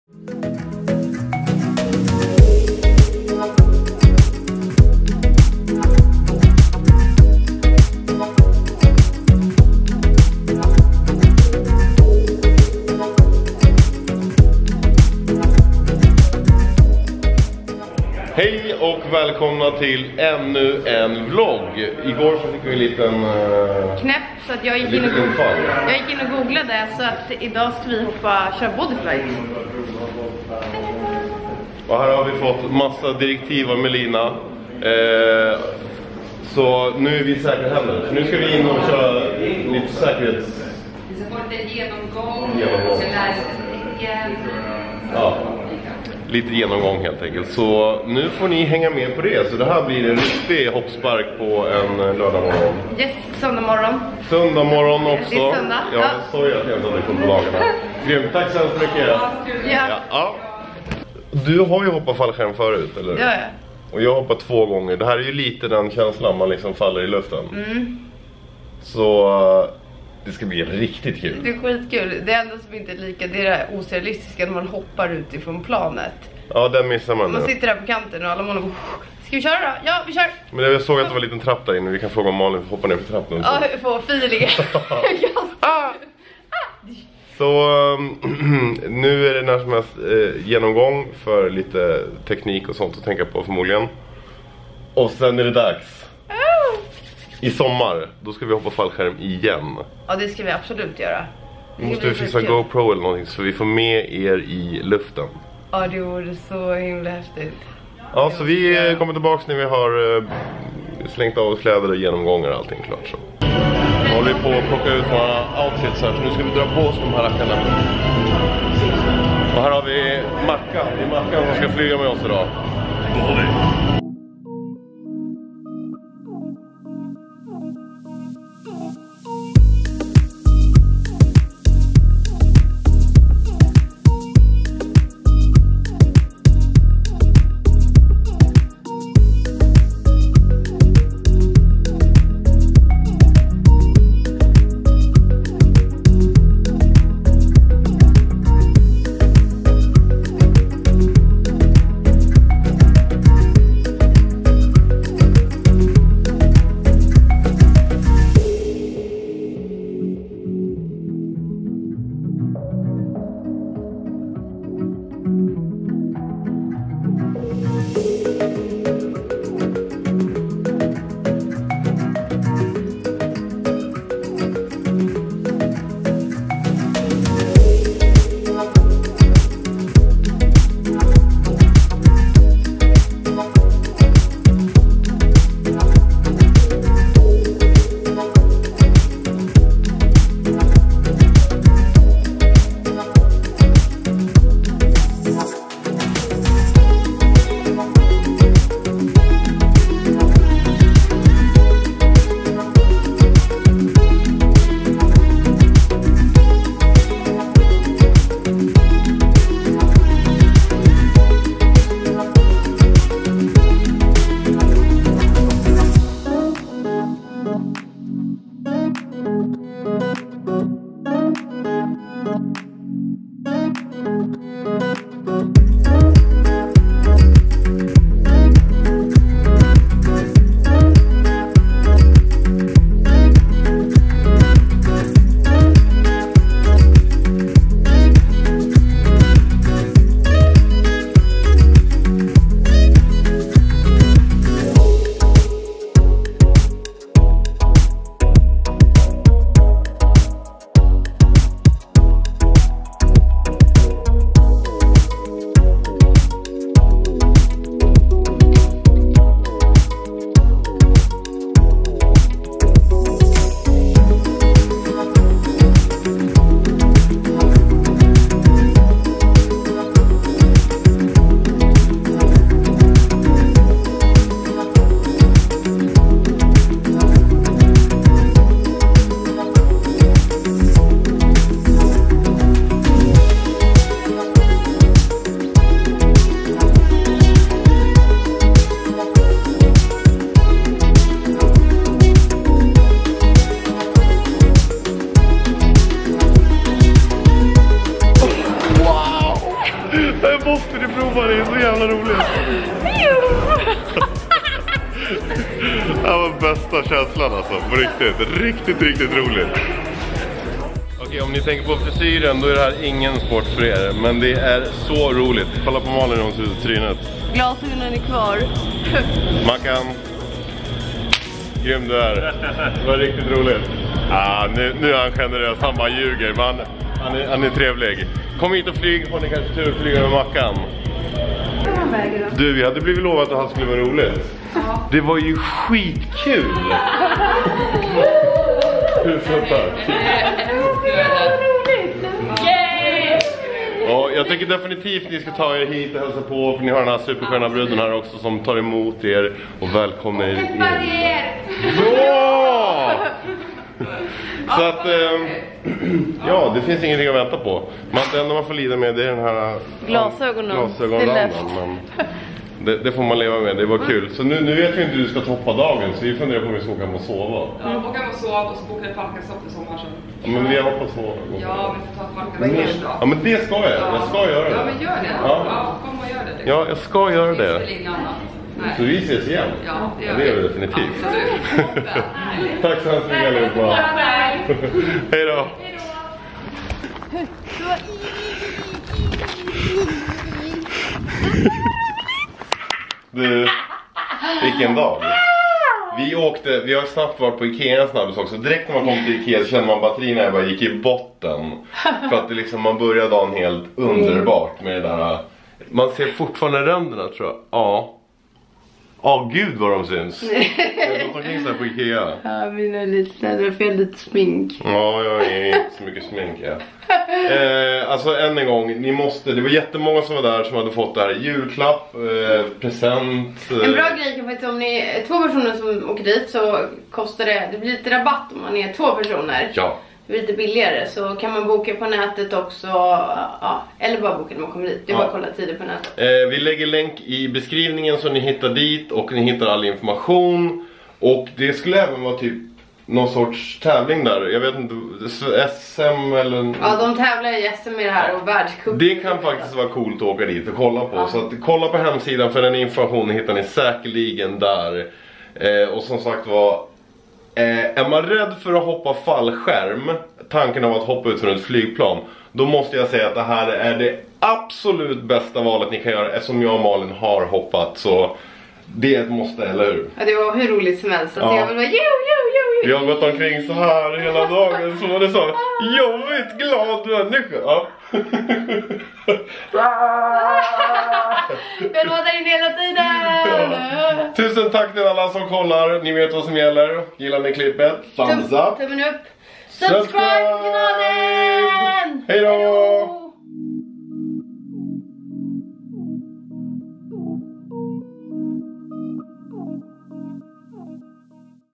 BODYFLIGHT STOCKHOLM
En söndag som vi kommer att minnas, ett extremt roligt och flygande besök på BODYFLIGHT Stockholm ute i Bromma.

Ett måste för er som inte testat så och boka en tid och flyg iväg!
Om ni är två så blir det något billigare för er.
In på hemsidan och boka din tid!